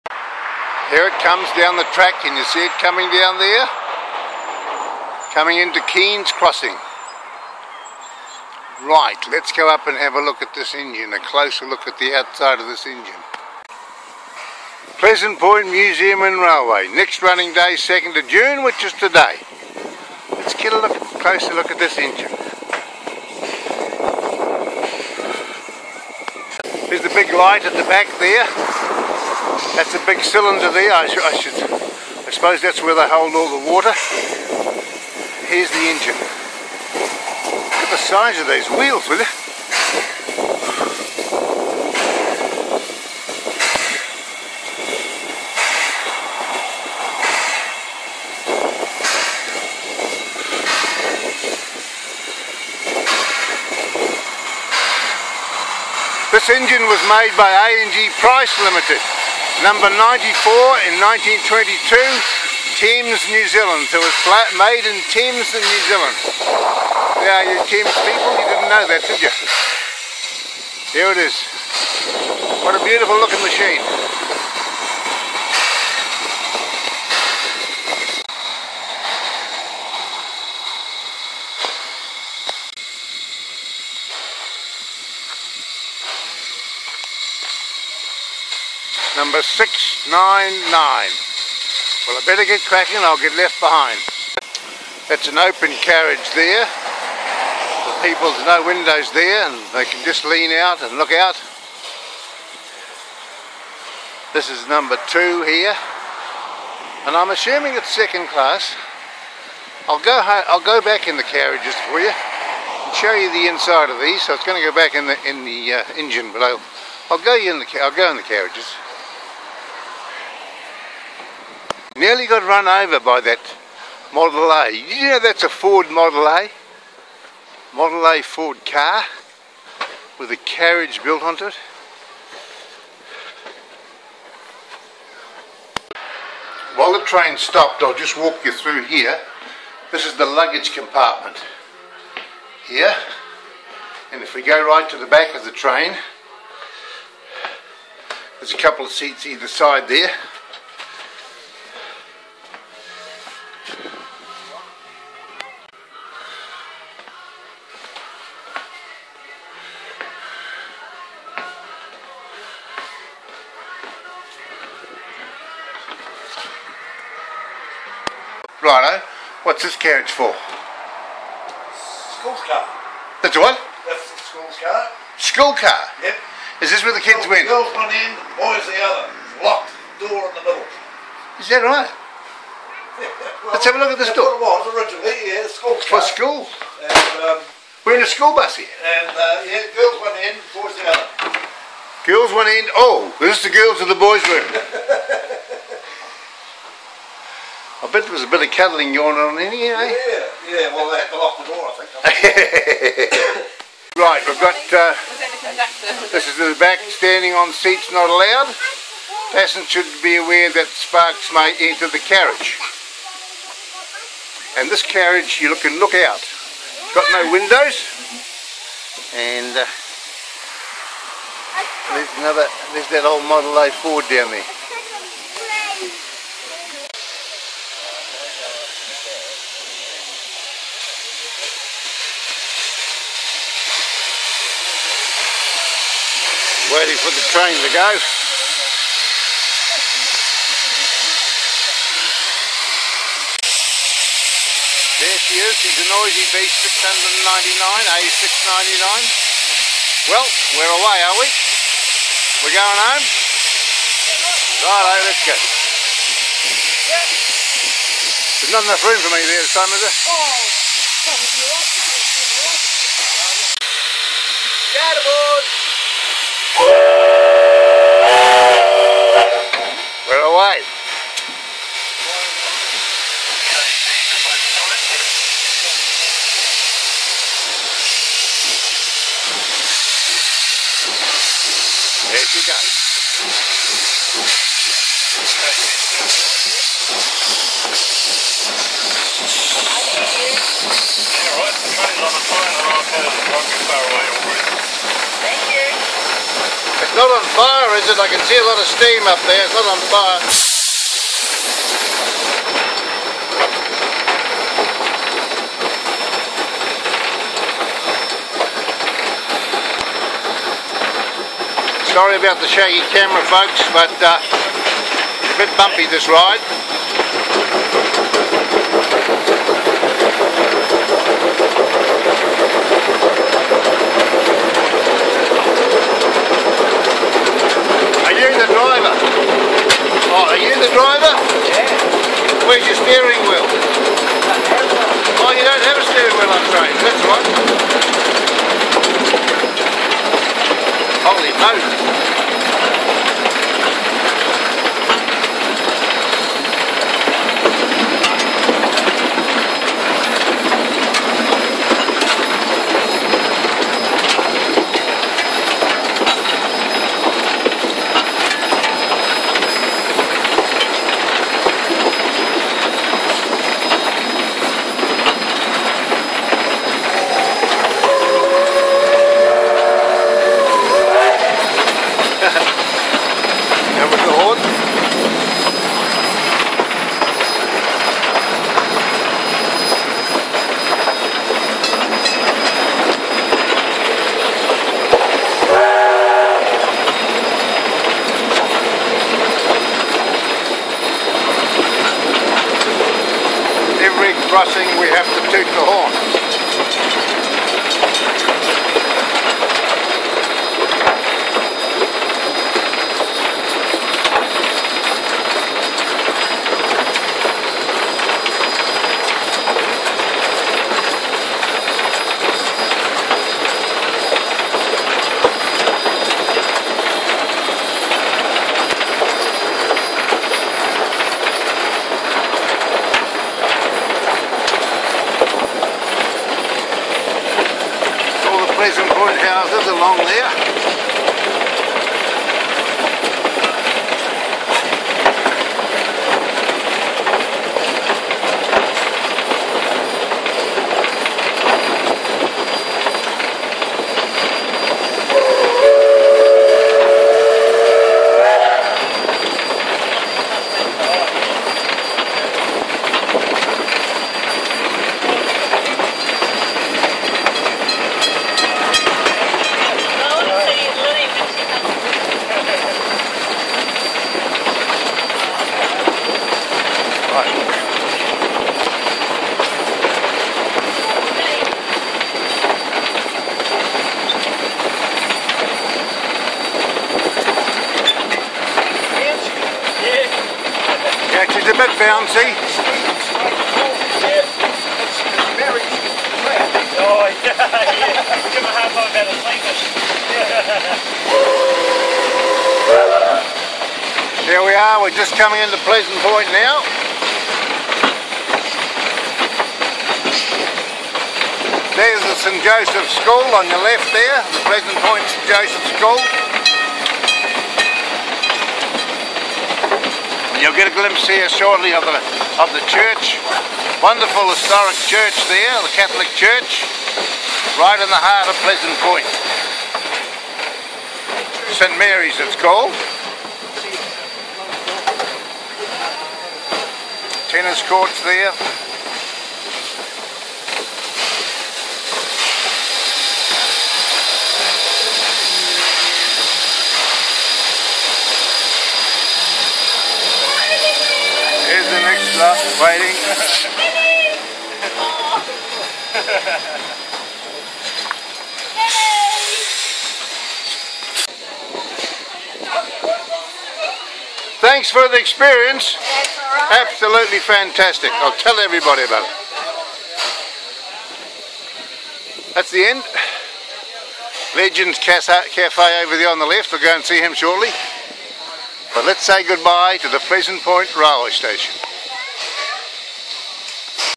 Pleasant Point N.Z. Railway & Museum - Part 4 Engine Ride home
A close up look at the Steam Engine No 699 and it carriages. Tourism Promoter Ron E Bishop gets a bumpy ride in the engine on its journey home to the Pleasant Point Railway Station.